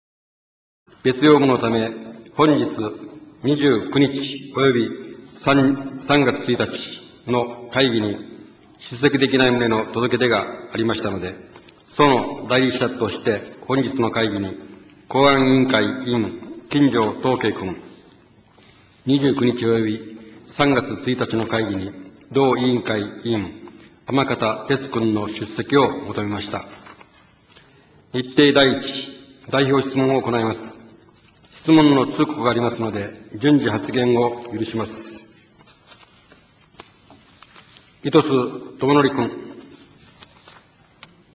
【與儀弘子】警視庁機動隊を要請し続ける沖縄県公安委員会委員長2/25 所用を理由に県議会出席拒否！
2/25 沖縄県議会  警視庁機動隊を要請し続ける與儀弘子・沖縄県公安委員会委員長　要請された沖縄県議会への出席を所用を理由に拒否！